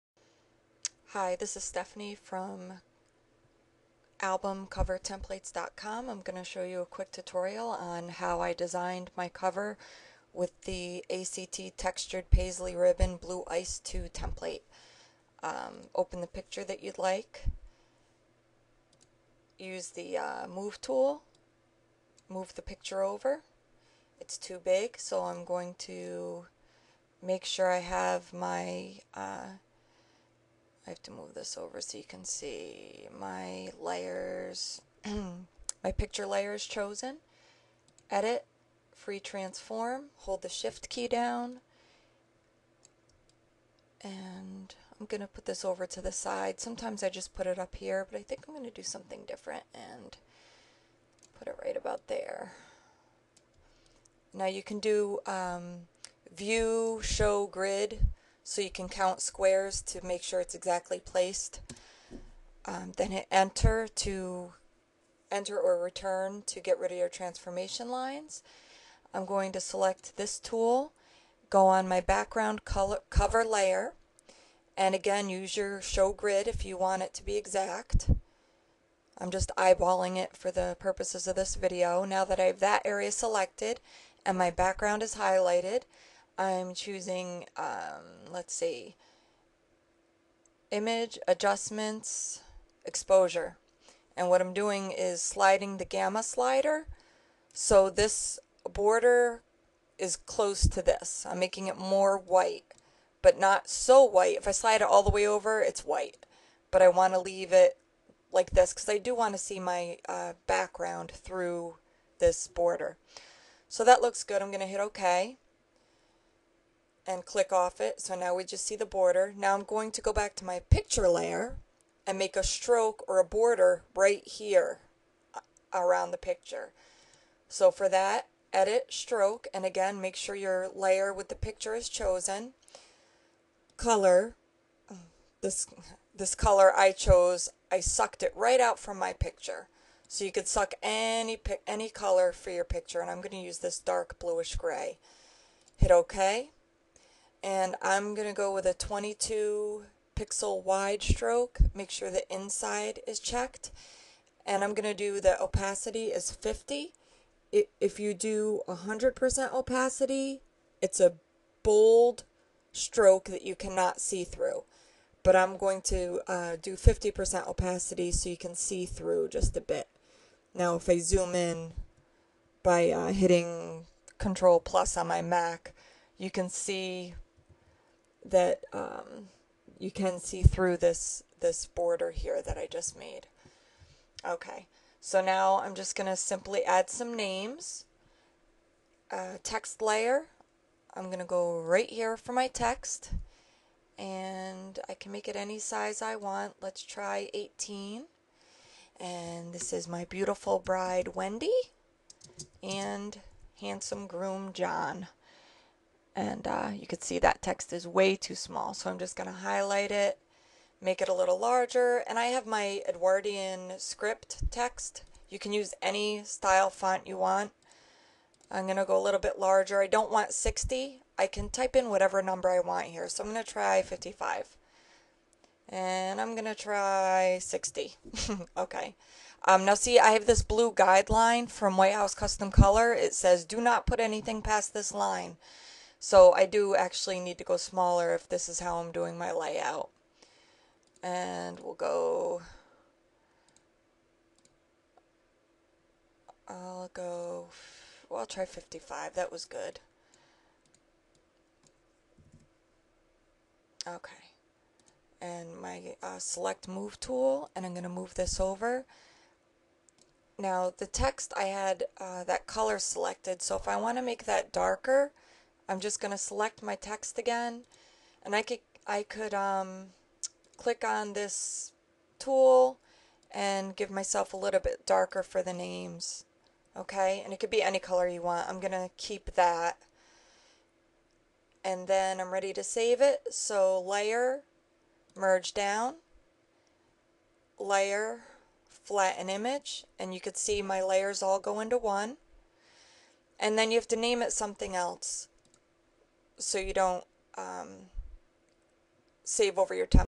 Tutorial Photoshop album cover design layout spread
Learn how to create custom covers using my templates.

SOOOOO EASY